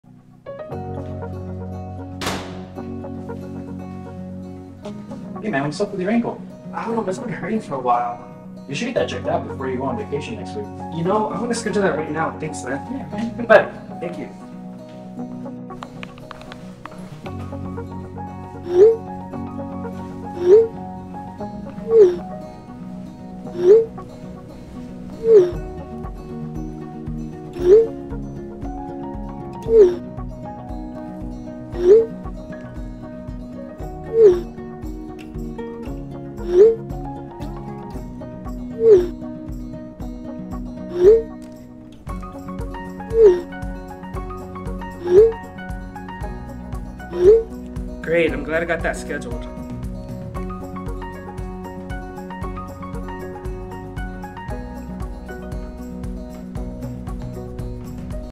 Schedule Appointments On Your Phone!
Schedule Your Appointments On Your Phone! 📱

We made scheduling your next appointment quick and easy! Don’t let an injury keep your from being active and healthy. You can make an appointment before or during your workout!

Get the care you need, when you need it.